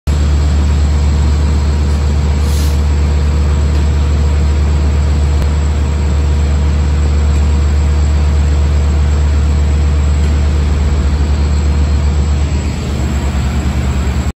Quick little clip I managed to get of the sperry service rail car
Sorry the video is quite short, I was so distracted by the beautiful de30ac up until I saw this coming around the corner and just barely managed to catch it...